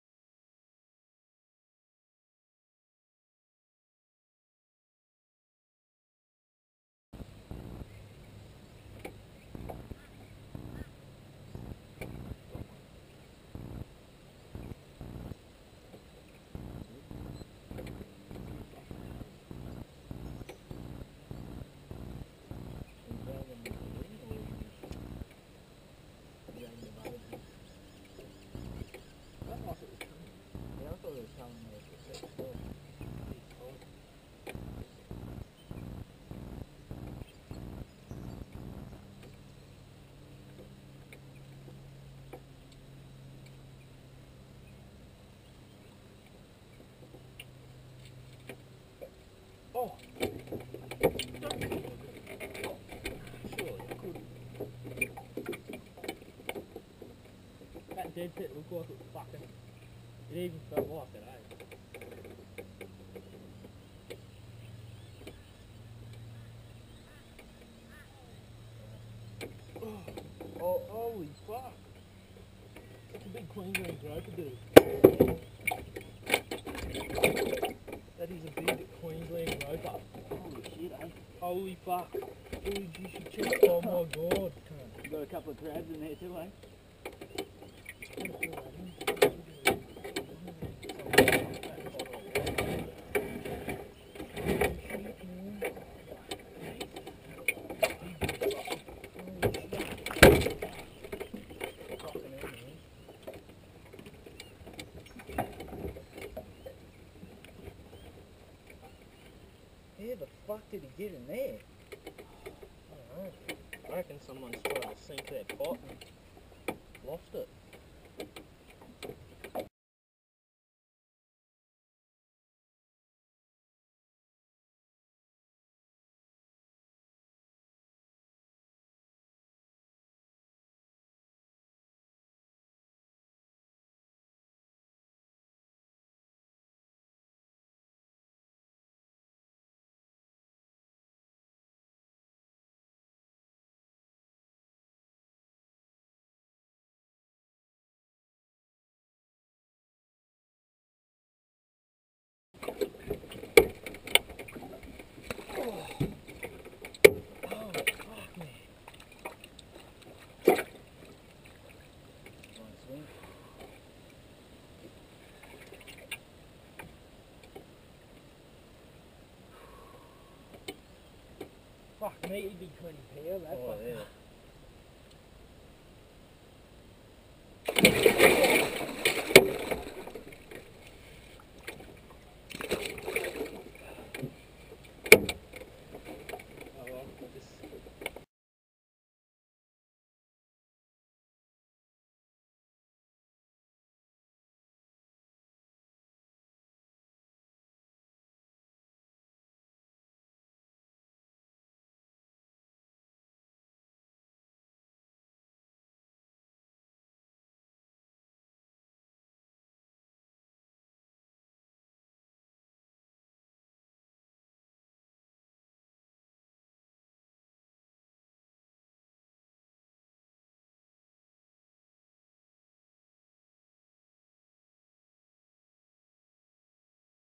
Queensland grouper stuck in a sunken crab pot
snagged a crab pot today. and pulled it up to find a queensland grouper stuck inside. the fish was in really poor condition and had scars (new ands old) over its body and on its fins. some of the scars gave the indication that it had been in the pot for quite some time. we opened the pot and carefully released him back into the water. and placed the pot up near the bank in the hope a passer buyer would pick up and dispose of the pot